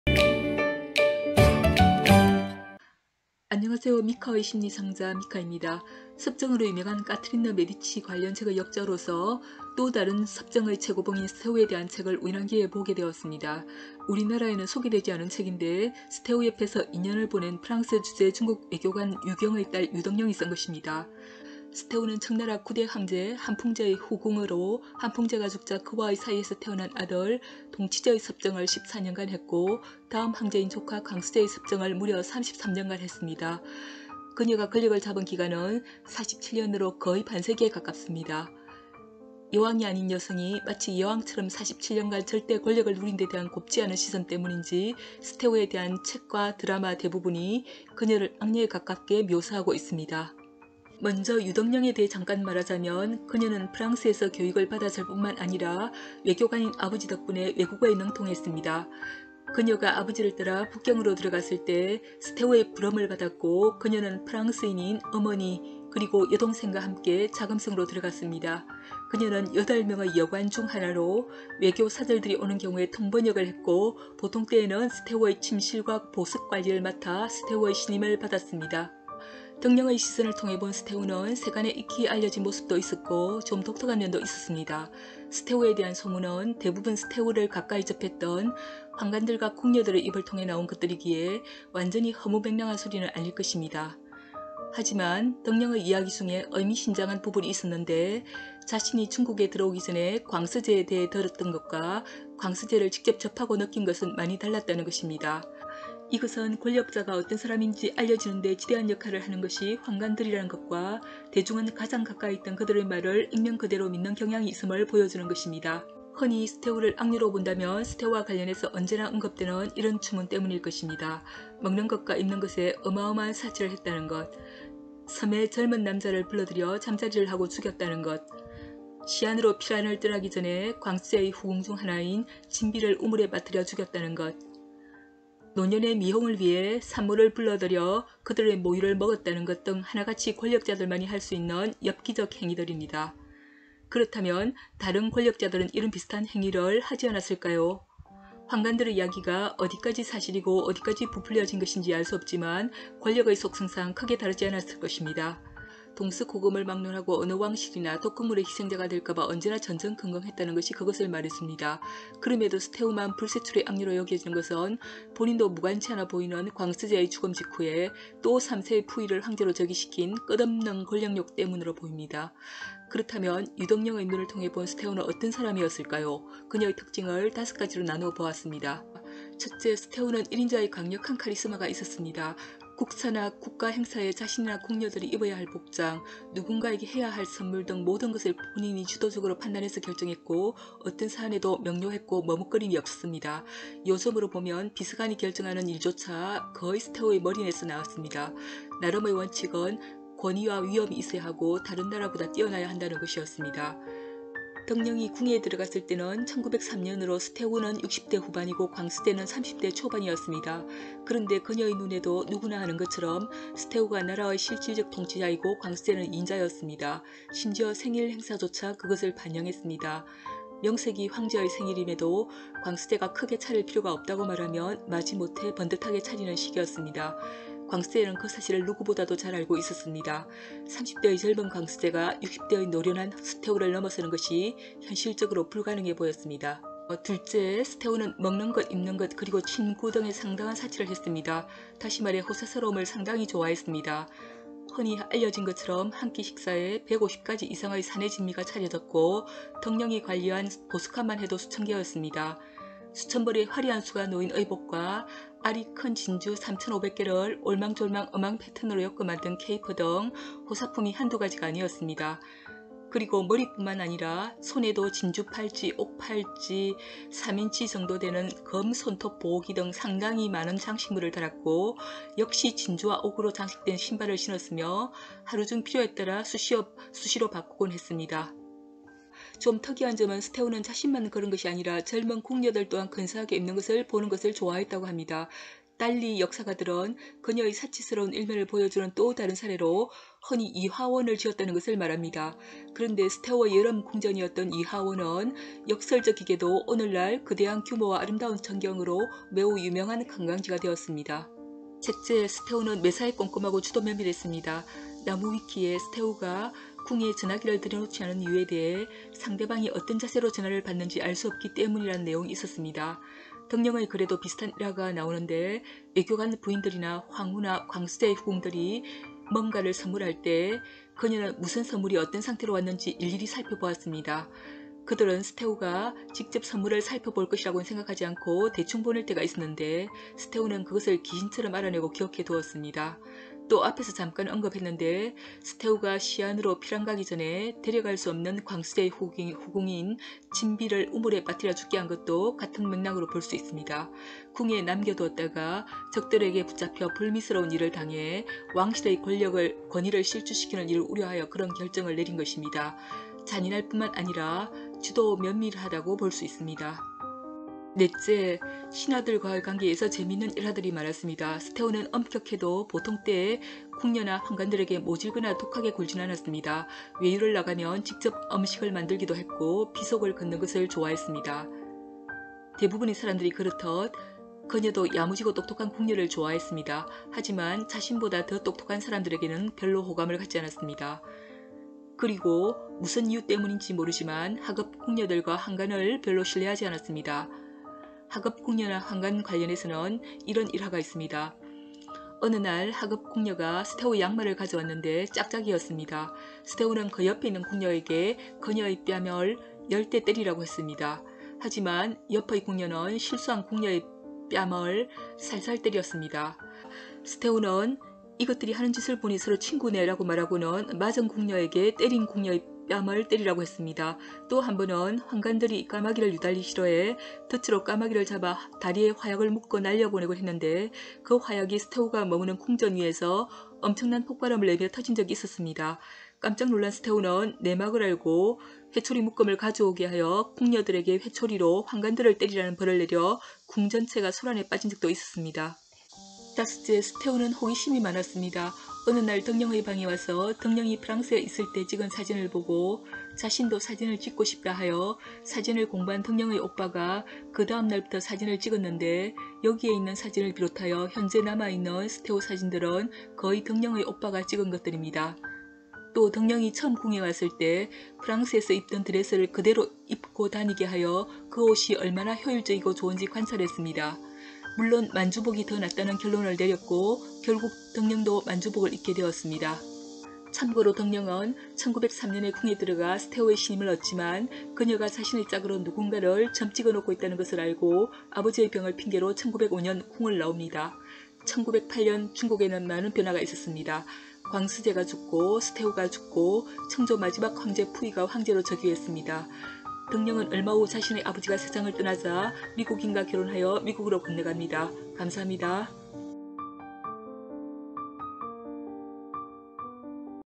프랑스 주재 중국 외교관 딸 유덕령이 2년간 접한 서태후는 어떤 여자였을까?
유덕령은  프랑스에서 교육을 받았을 뿐만 아니라 외교관인 아버지 덕분에 외국어에 능통했습니다. 그녀가 아버지를 따라 1900년대 (1903년에)북경으로 들어갔을 때 서태후의 부름을 받았고 그녀는 프랑스인인 어머니, 그리고 여동생과 함께 자금성으로 들어갔습니다. 그녀는 8명의 여관(여성 관료) 중 하나로 외교 사절들이 오는 경우에 통번역을 했고 보통 때에는 서태후의 침실과 보석 관리를 맡아 서태후의 신임을 받았습니다.  그 곳에서 그녀가 2년간 접한 서태후는 어떤 여자였을까요?